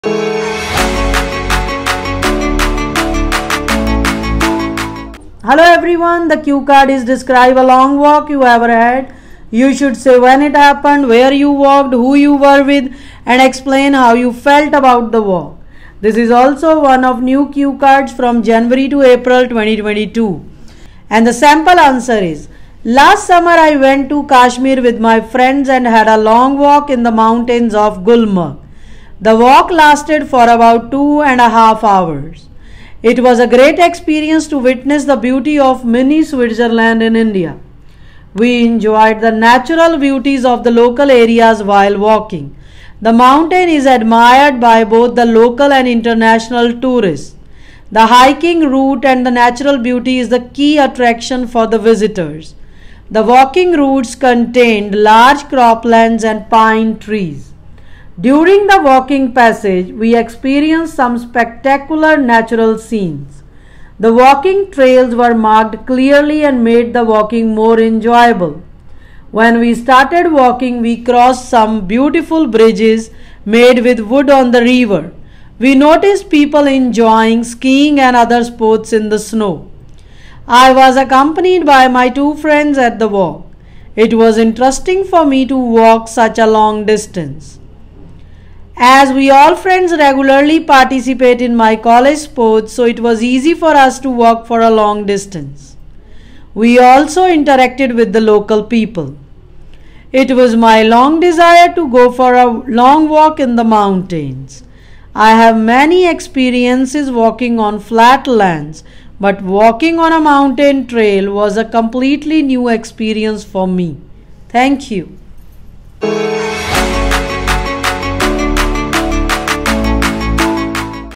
Describe a long walk you ever had
You should say:
when this happened
where you walked
who you were with
and explain how you felt about this long walk

Our Key features are:
• Personalized Coaching
• Experienced